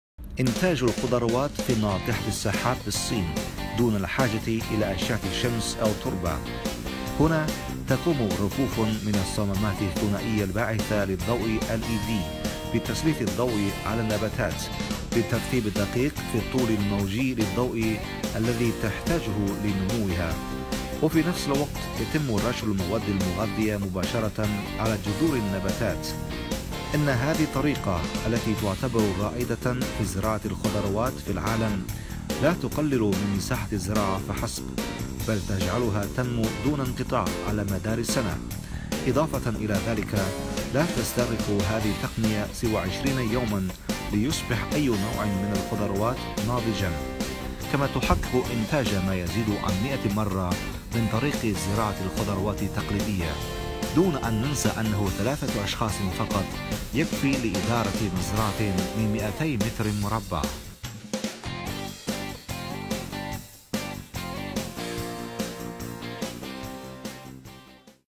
الإبداع الصيني: إنتاج الخضروات في ناطحة سحاب بالصين دون الحاجة الى أشعة الشمس أو التربة!!!
خلافا لانتاج الخضروات التقليدية تستخدم الصين الضوء بتقنية(LED) بدلا من أشعة شمس والتربة لتجعل النباتات تنمو في المزرعة المغلقة حتى ولو في ناطحة سحاب.
هنا تقوم رفوف من الصمّامات الثنائية الباعثة للضوء (LED) بتسليط الضوء على النباتاتب بالترتيب الدقيق في الطول الموجي للضوء الذي تحتاجه لنموها، وفي نفس الوقت يتم رش المواد المغذّية مباشرة على جذور النباتات. إن هذه الطريقة التي تعتبر رائدة في زراعة الخضروات في العالم، لا تقلل من مساحة الزراعة فحسب، بل تجعلها تنمو دون انقطاع على مدار السنة، اضافة الى ذلك لا تستغرق هذه التقنية سوى 20 يوما ليصبح اي نوع من الخضروات ناضجا، كما تحقق إنتاج ما يزيد عن مائة مرة من طريقة زراعة الخضروات التقليدية! دون ان ننسى انه ثلاثة أشخاص فقط يكفي لإدارة مزرعة من 200 متر مربع.